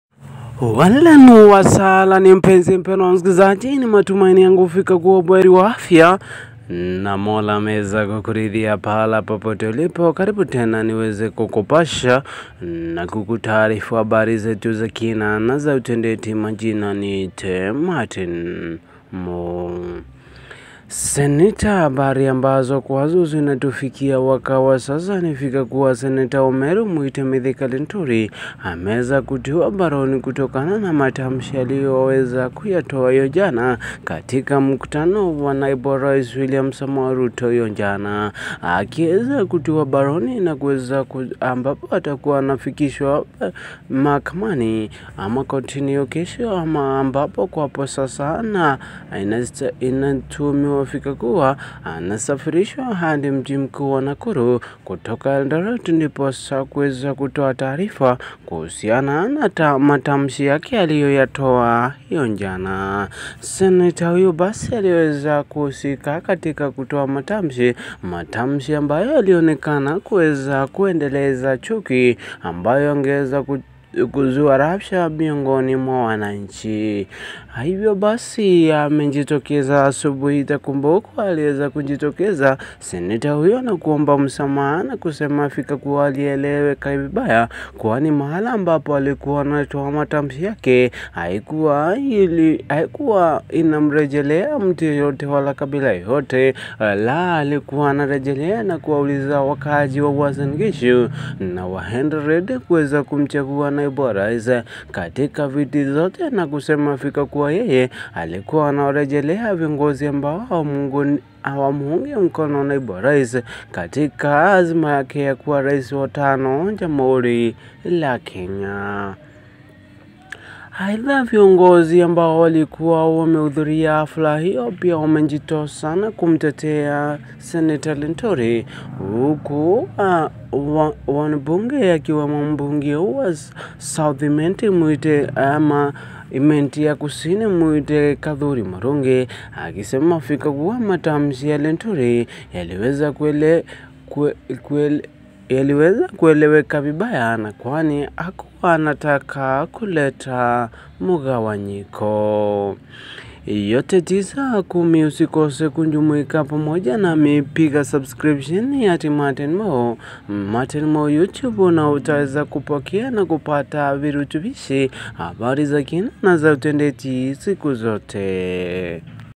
Sen. linturi arrested.
Meru senator mithika linturi arrested for utterances he made yesterday.